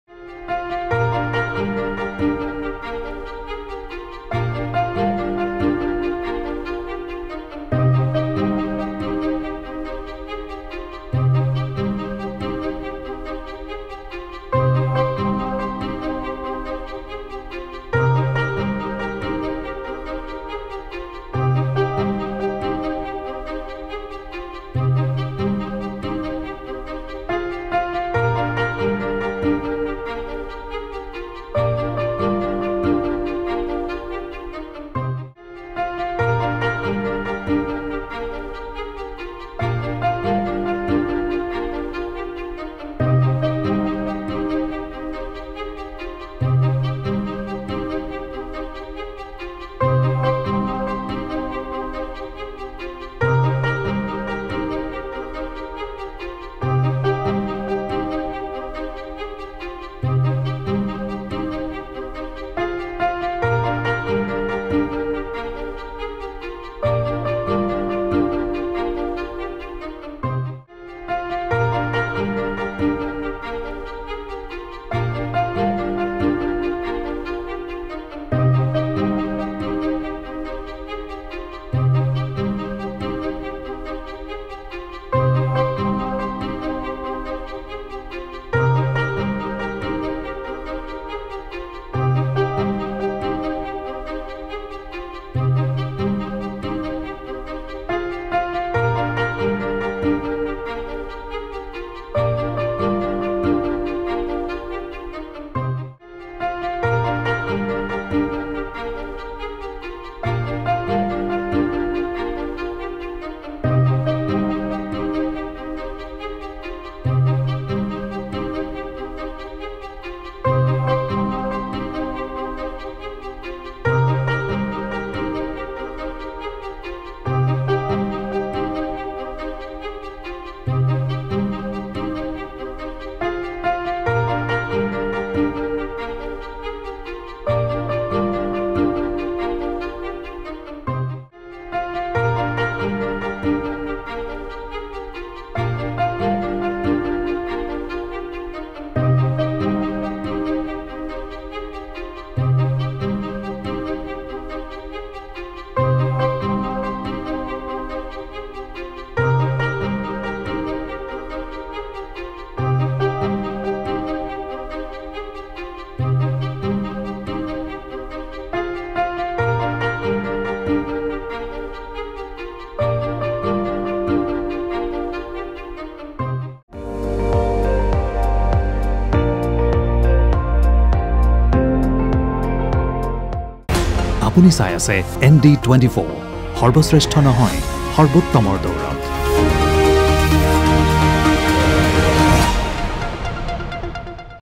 Ranjeet Kumar Dass News: তিনিচুকীয়াৰ গুণোৎসৱত ৰান্ধনীৰ ৰূপত মন্ত্ৰী ৰণজিৎ কুমাৰ দাস
তিনিচুকীয়াৰ গুণোৎসৱত ৰান্ধনীৰ ৰূপত মন্ত্ৰী ৰণজিৎ কুমাৰ দাস